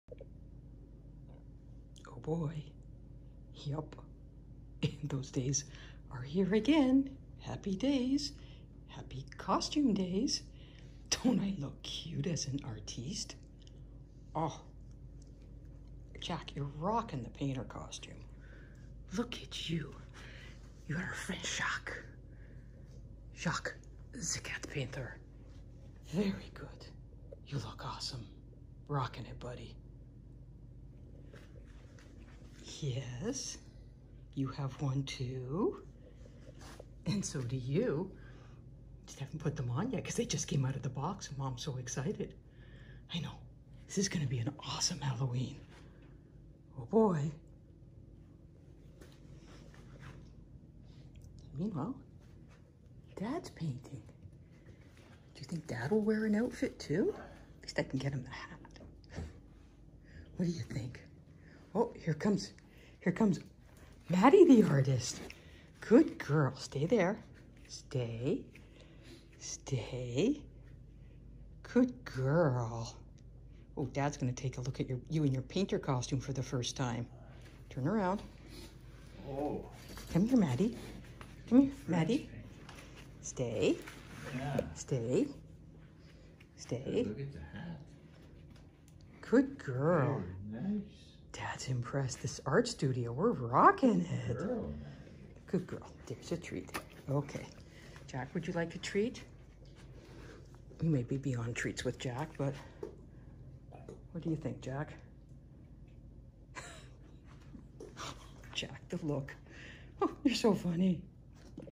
Funny Cat & Great Dane Model French Painter Outfits To Artist Dad
Maddie and Jack the Cat love to relax in the art room while Dad works on his latest horse painting.  What better way to show their support for Dad's masterpiece than get dress up with their own palettes.  Watch and laugh as Jack and Maddie model their new Parisian Painter Costumes and enjoy Dad's reaction to his budding artists. Don't worry Ellie, there is another costume for you too, when you get up from your nap!